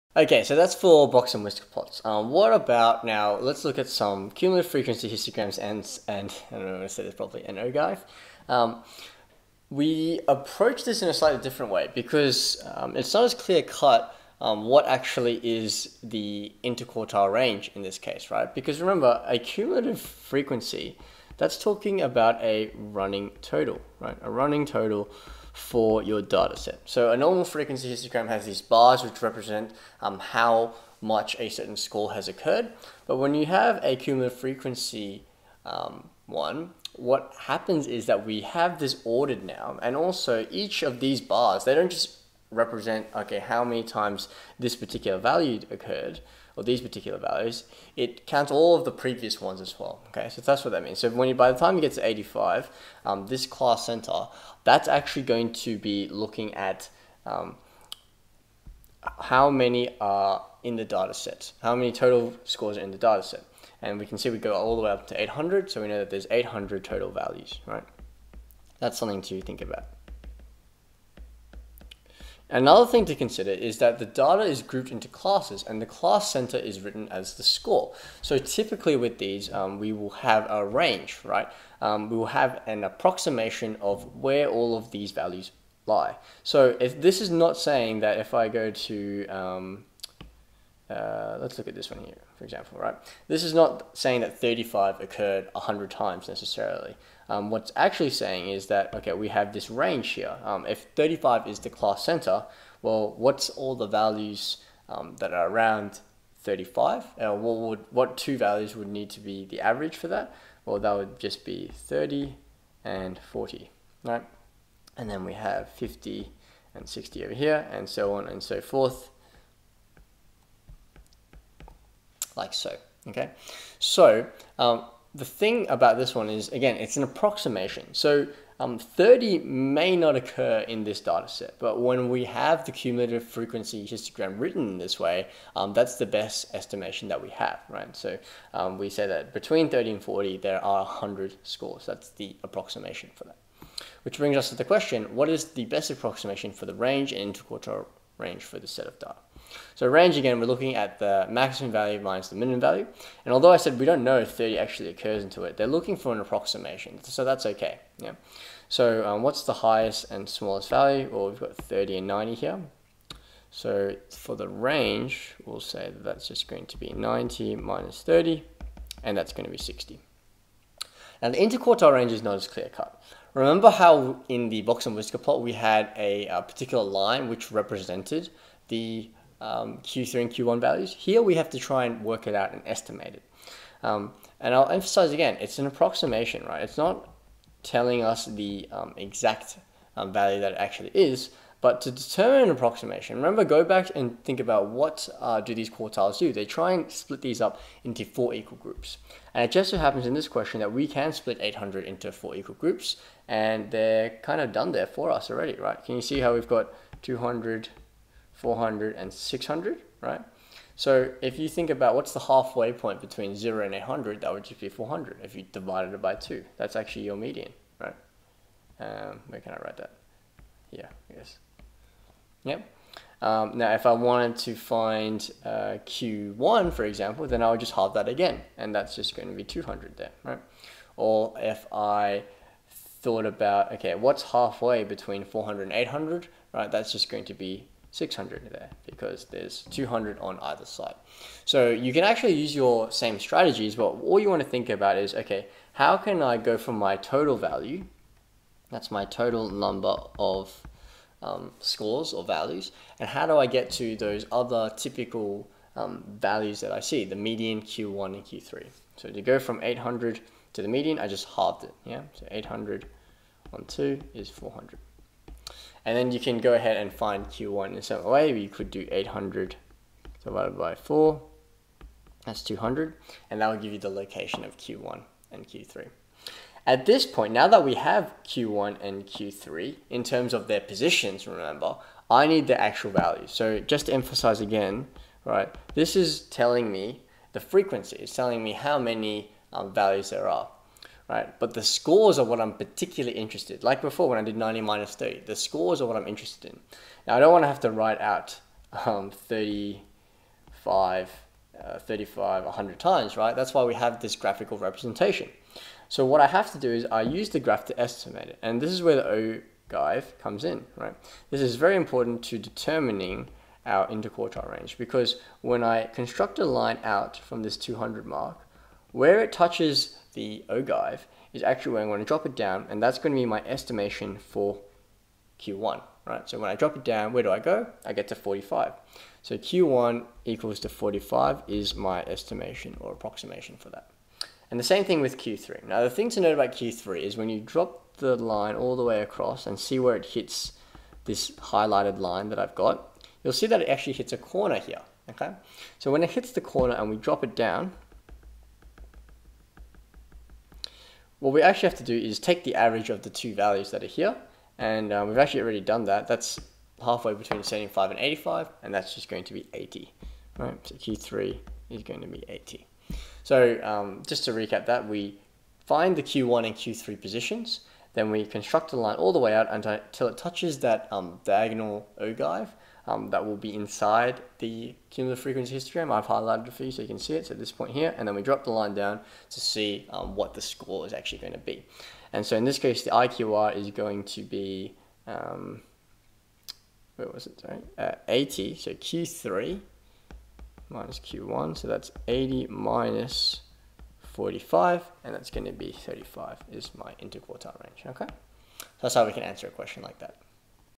Range and Interquartile Range (Part 2: Cumulative Frequency Histogram & Ogives)
How to use cumulative frequency histograms and ogives to solve problems on range and interquartile range.